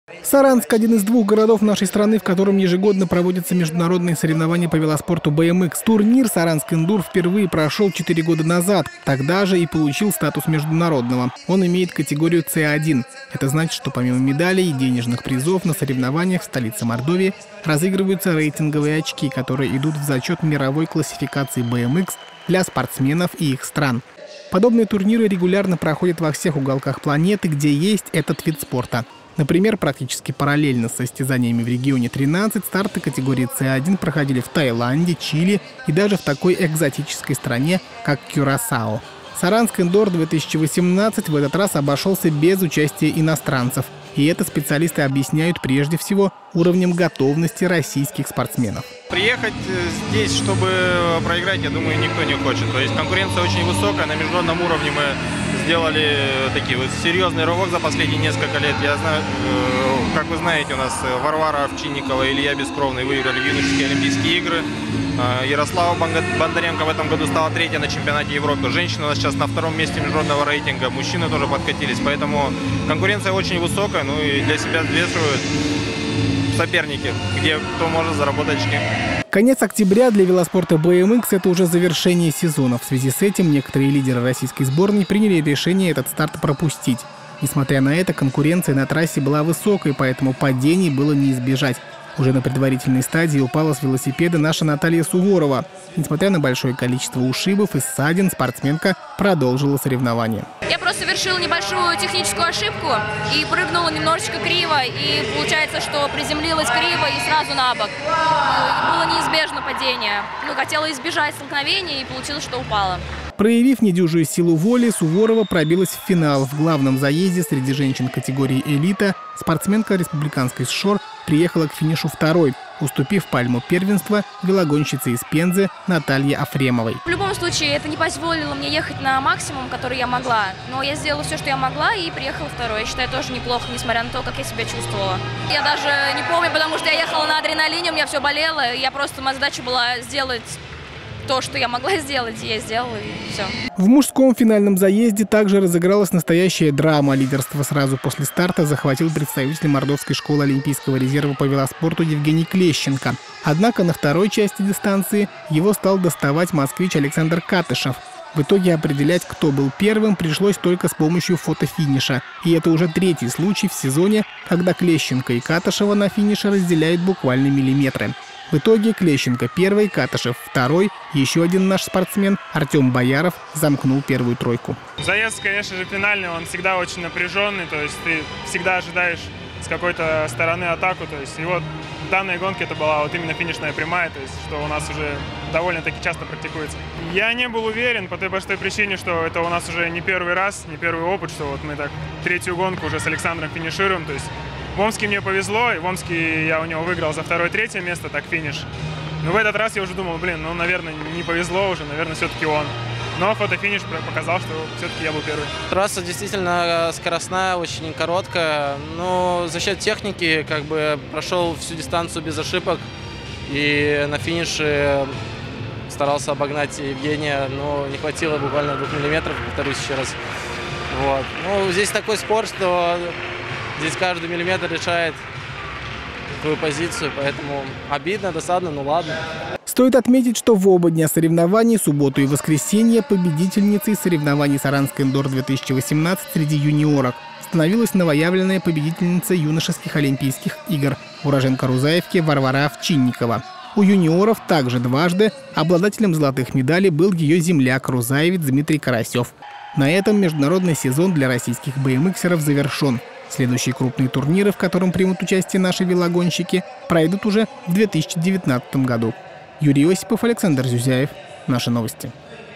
«Saransk Indoor 2018»
В Саранске в течение двух дней проходили международные соревнования по велоспорту ВМХ «Saransk Indoor 2018». Турнир имеет категорию С 1. Это значит, что помимо медалей и денежных призов на соревнованиях в столице Мордовии разыгрываются рейтинговые очки, которые идут в зачет мировой классификации ВМХ для спортсменов и их стран.